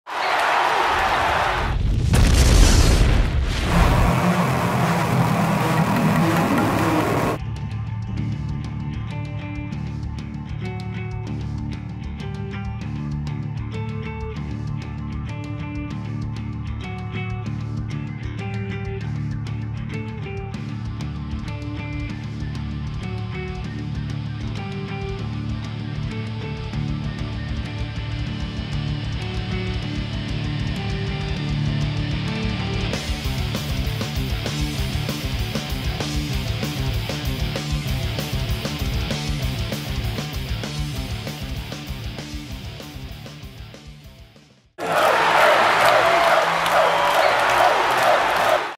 PPM Festival - New band's announcement - 20.12.13
New band announced: Fates Warning